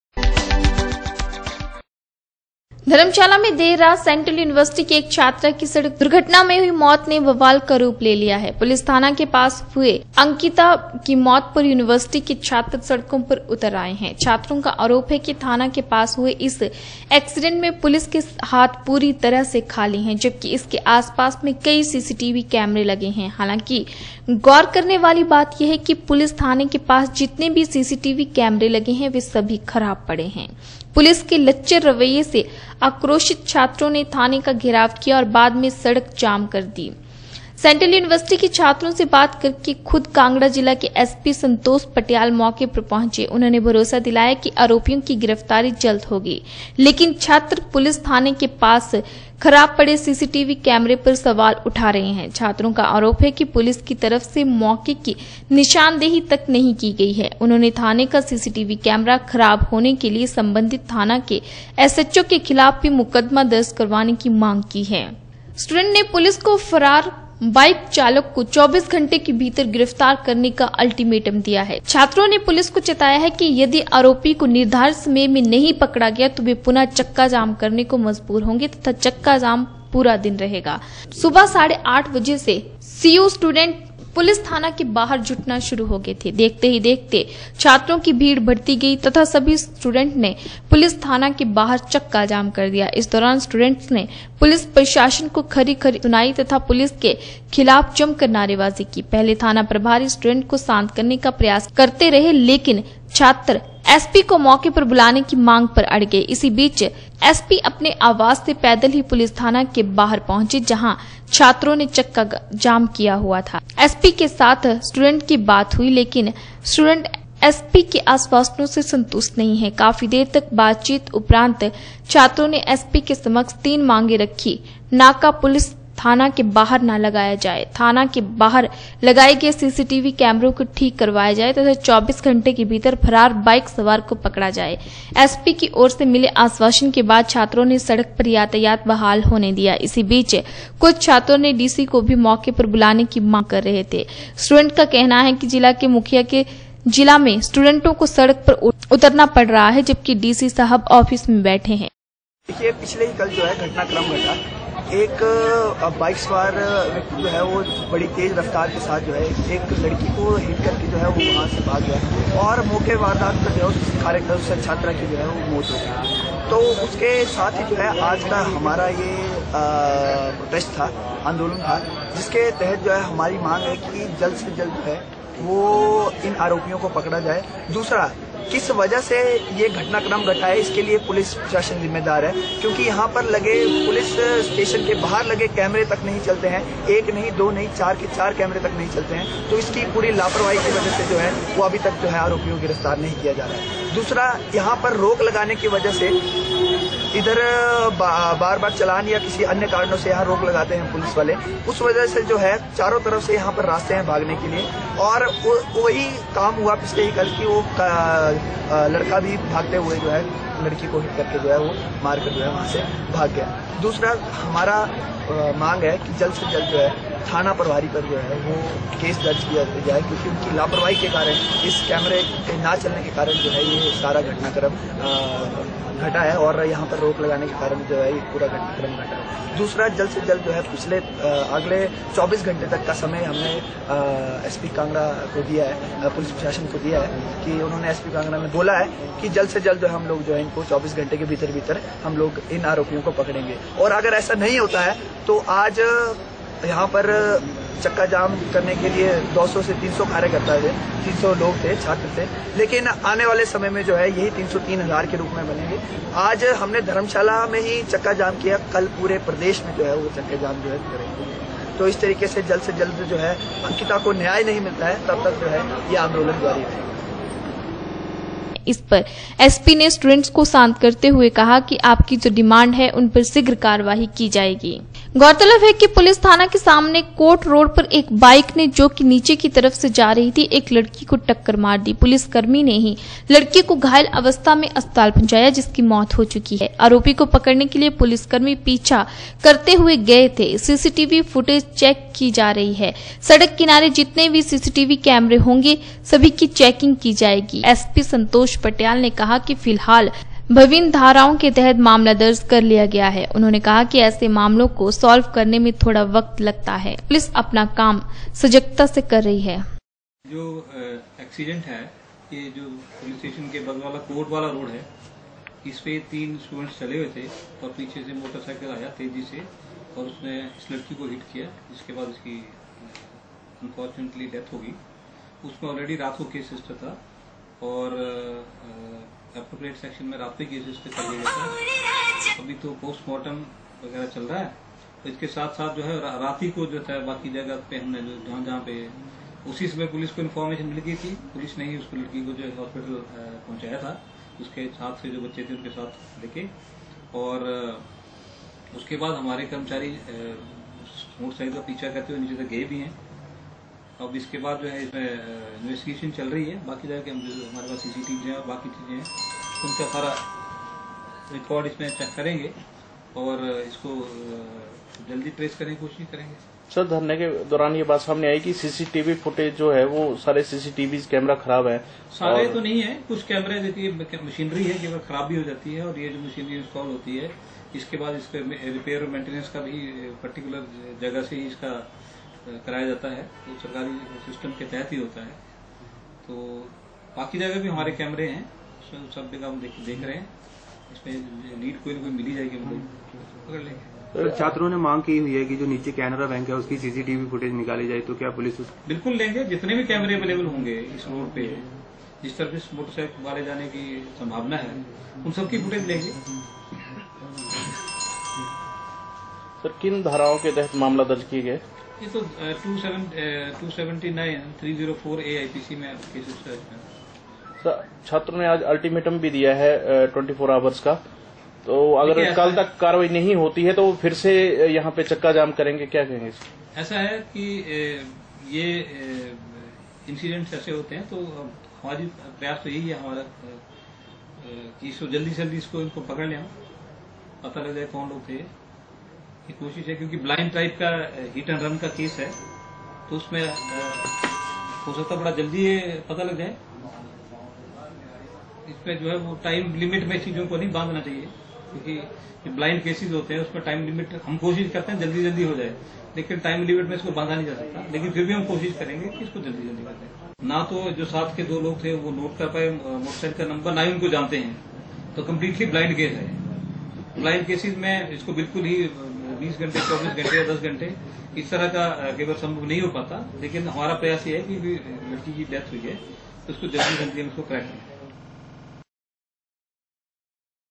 सेंट्रल यूनिवर्सिटी की एक छात्रा की सड़क दुर्घटना में हुई मौत ने बवाल का रूप ले लिया
धर्मशाला में देर रात सेंट्रल यूनिवर्सिटी की एक छात्रा की सड़क दुर्घटना में हुई मौत ने बवाल का रूप ले लिया है। पुलिस थाने के पास हुए अंकिता (बिलासपुर) की मौत पर यूनिवर्सिटी के छात्र सड़कों पर हैं। छात्रों का आरोप है कि थाने के पास हुए इस एक्सिडेंट में पुलिस के हाथ पूरी तरह खाली हैं। जबकि, इसके आसपास में कई सीसीटीवी कैमरे लगे हैं।